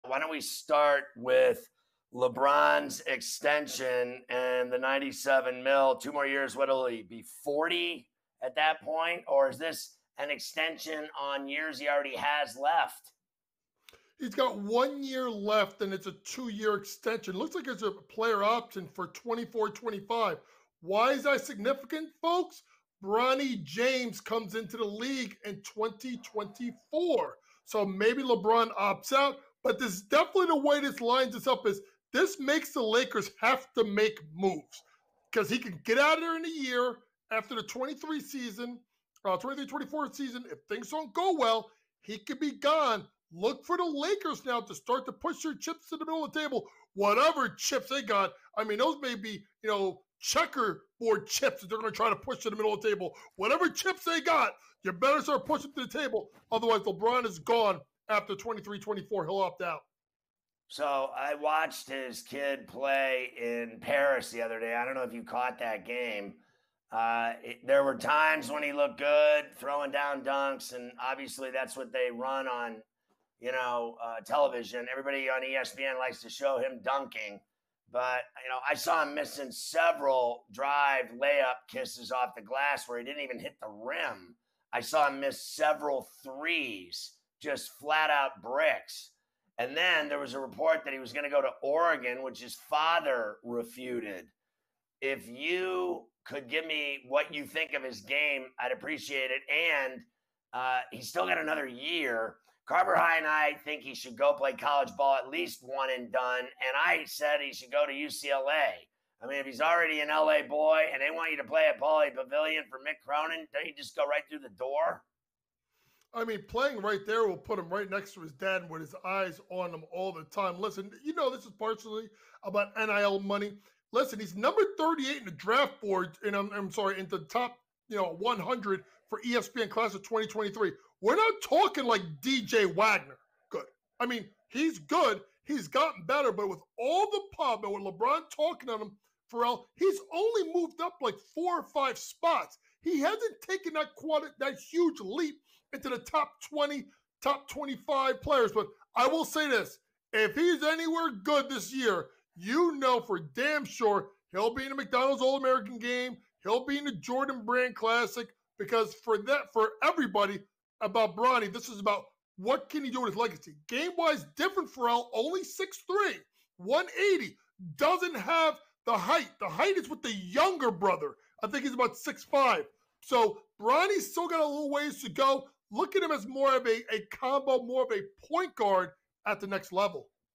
LeBron James Signs 2-Year Extension With Lakers
Scott Ferrall and James Young discuss LeBron James.